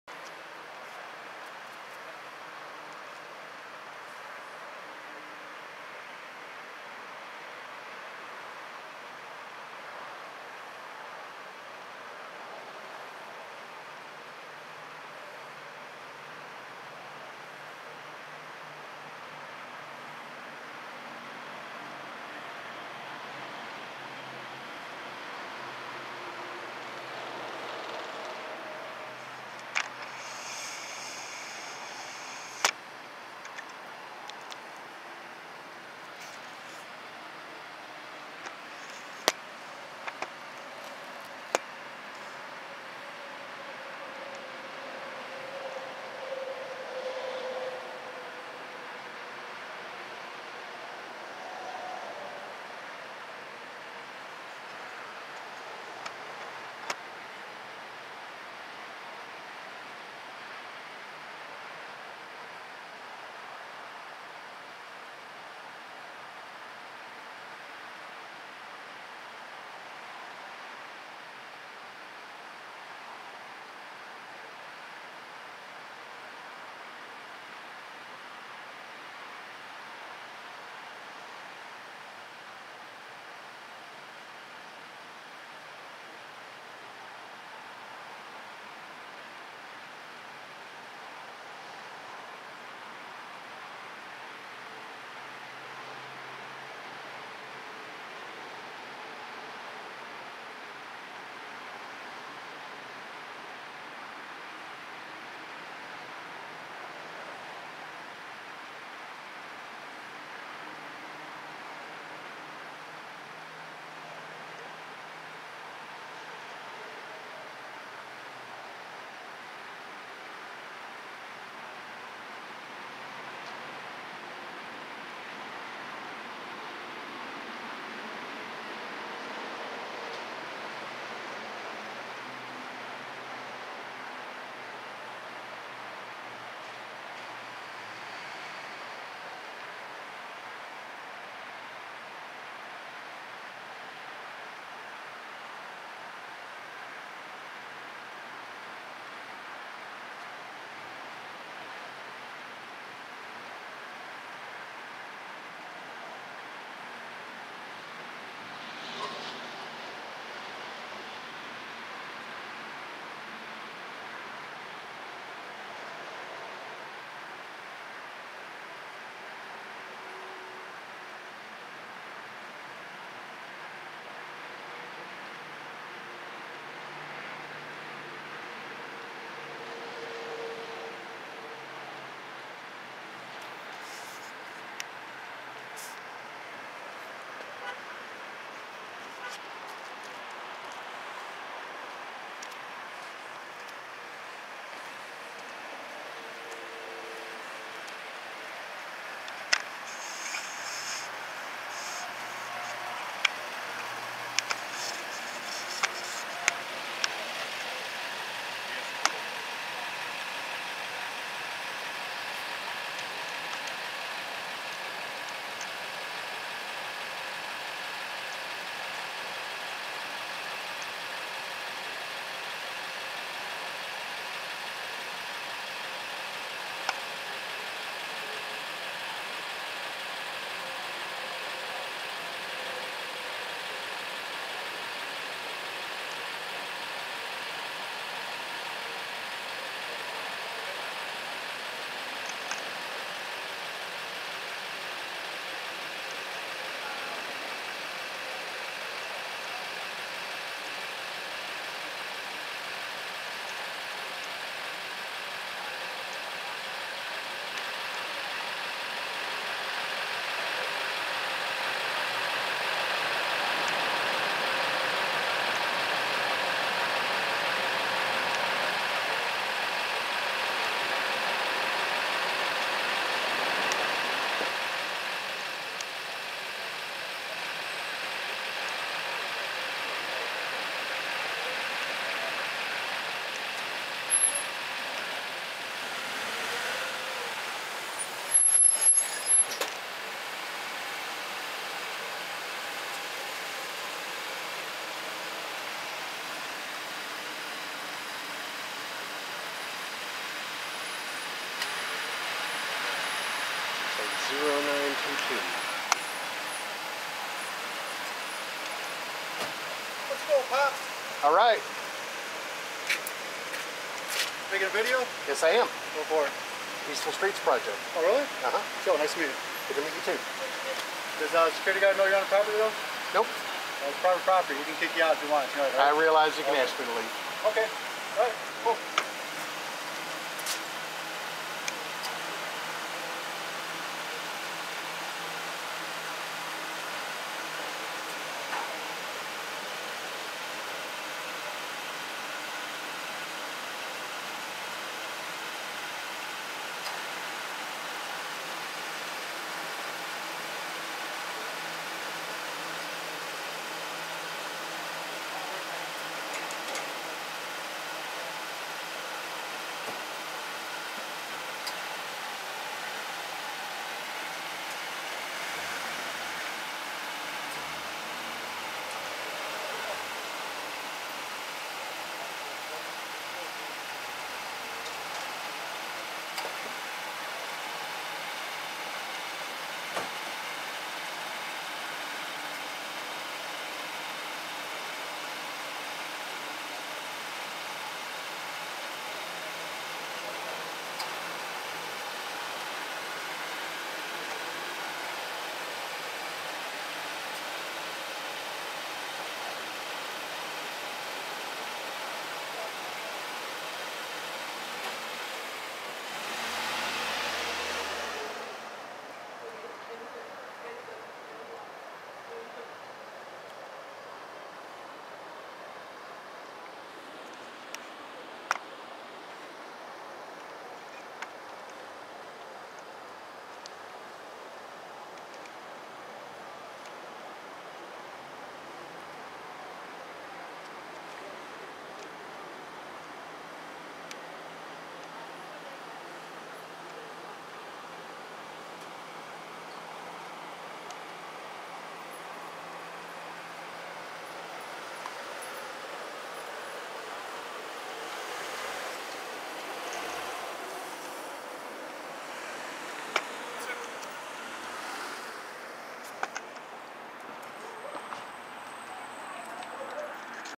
Austin Police and TxDPS - Incident at Motel 6 - Rundberg & I-35 - 5/10/2014 12:05AM
I don't know what started all of this.  I obviously arrived as it was clearing.  At 5:10 into the video, one of the APD officers asked why I was filming.  A bit later, on the "Police Scanner" app, I heard them mention the "Peaceful Streets Cameraman," with my description.